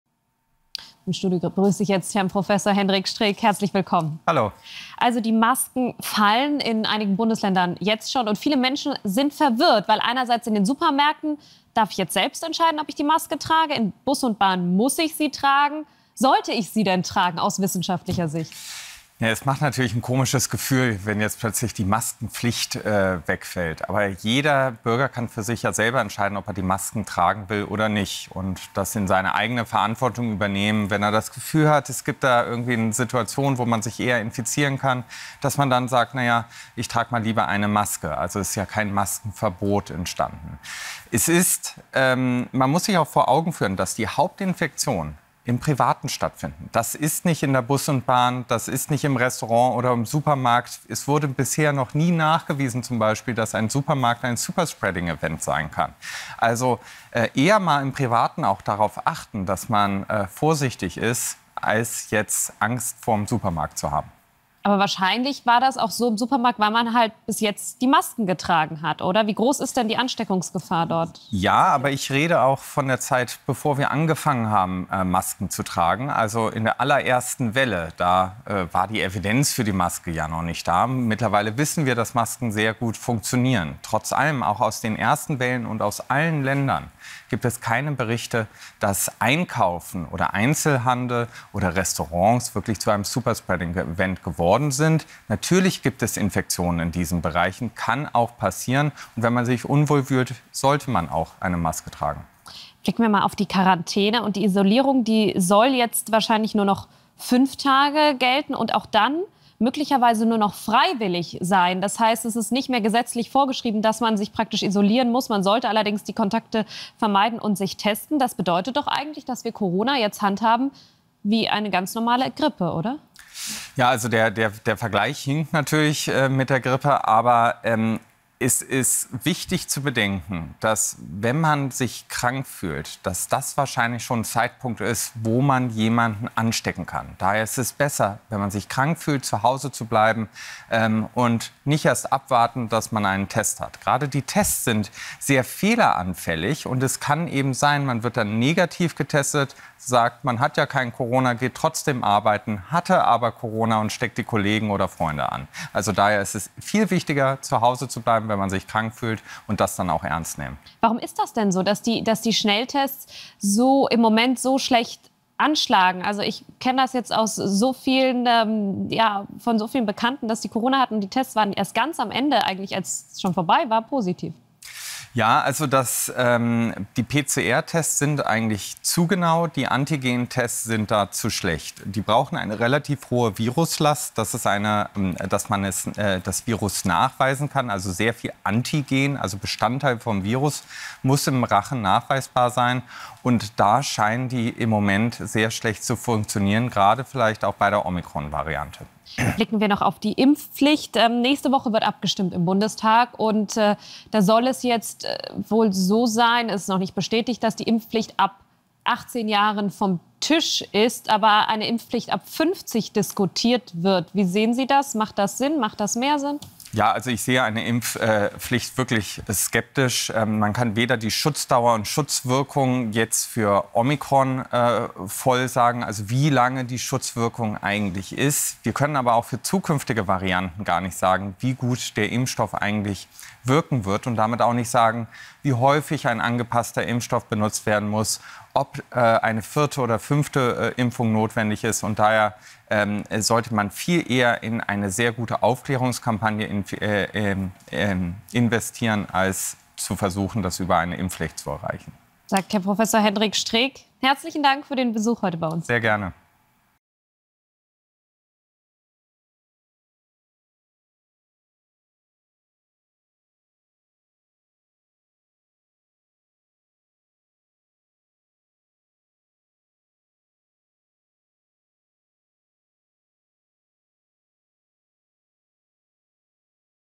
CORONA: "Superspreading-Event"? Was bisher noch nie nachgewiesen wurde - Hendrik Streeck
Die Deutschen lassen ihre Maske fallen, denn die Corona-Beschränkungen enden weitestgehend. Doch wird nun die Virus-Infektion genauso behandelt werden wie eine normale Grippe? „Es ist besser, wenn man sich krank fühlt, zu Hause zu bleiben“, sagt Virologe Hendrik Streeck.